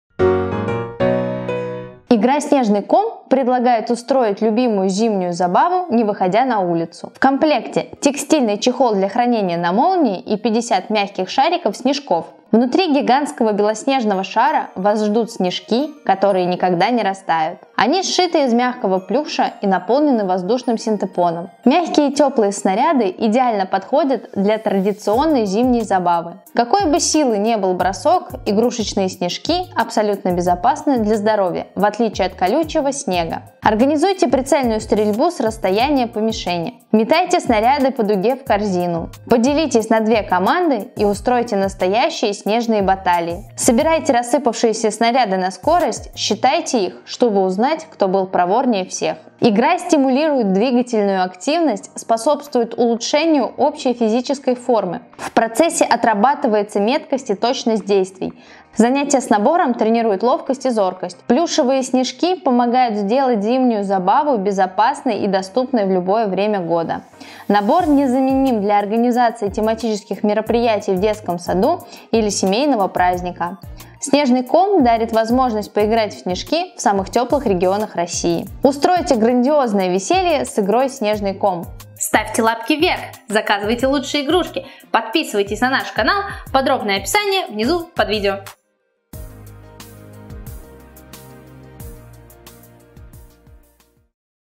Игра «Снежный ком» (50 снежков), Вини
Игра Снежный ком приглашает всех принять участие в любимой зимней забаве – прицельной стрельбе снежками. Вот только импровизированные белые комочки – теплые и мягкие. Устройте веселье для детей, не выходя на мороз!

Нам доверяют наши клиенты!